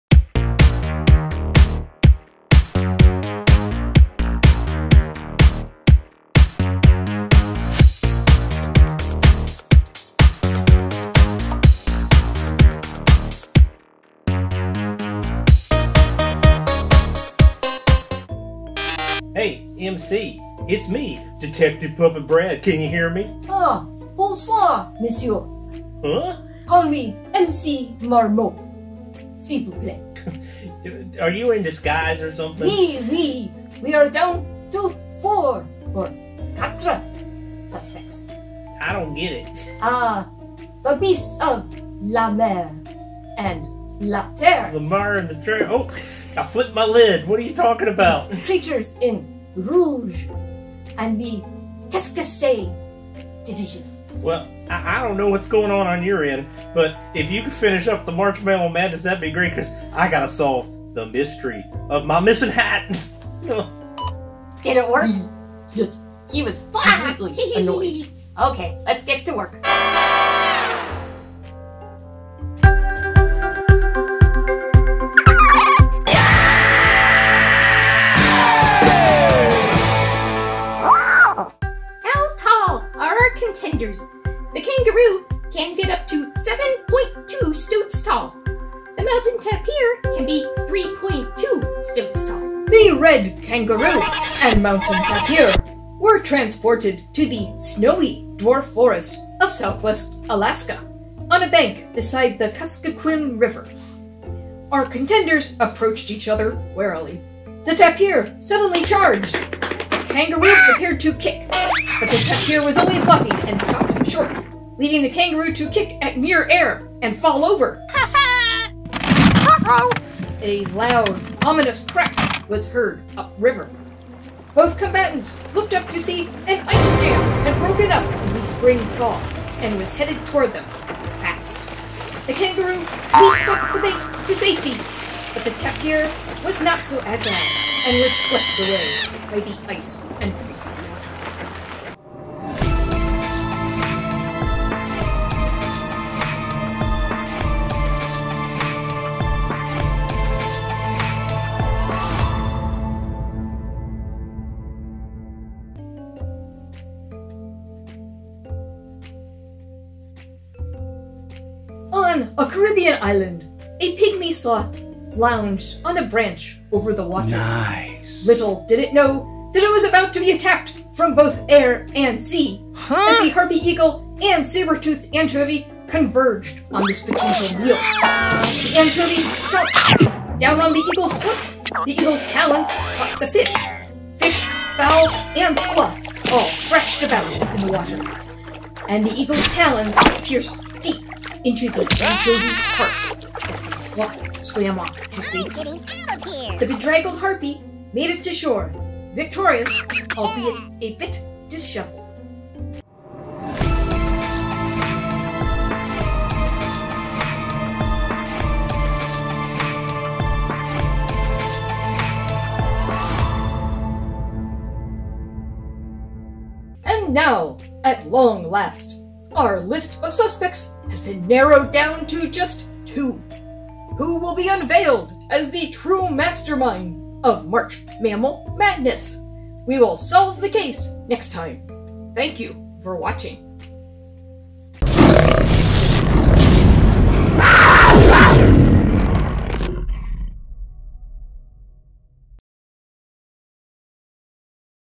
Rodent Recap - 2021 MMM - Final Roar!
Come along with the Rodent Roundtable as they recap and investigate March Mammal Madness in 2021! Tonight, it's the Final Roar!

(And a correction: Follow 2021MMM for all things Mammal Madness related; ignore the hashtag at the end. Rodent finger fumble, very sorry.)

Important information and links to follow:

- Science! for all things MMM and follow the official Twitter account There's a LOT more going on in these battles than we can recap in a few minutes.

- Art!

- Science AND Art and More!

- Stoats As Measurement! Figure out your own stoaticity with the Stoatally Awesome Converter

Due to YouTube's new policies, comments are no longer allowed.